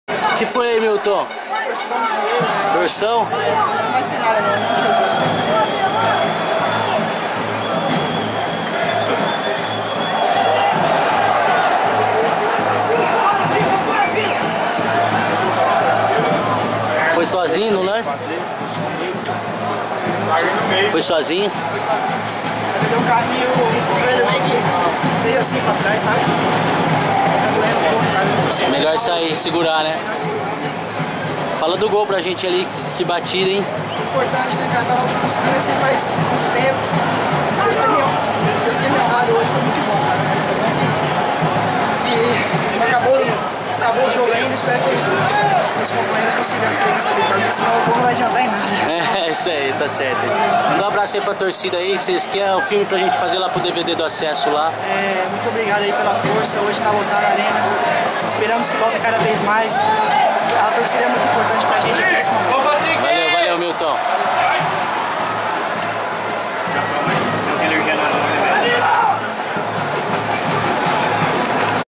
Especial do Acesso: Ferroviária 4 x 0 Novorizontino - Milton Junior fala logo após sair por lesão!
O volante, que marcou um golaço, sofreu uma entorse no joelho mas comemorou o resultado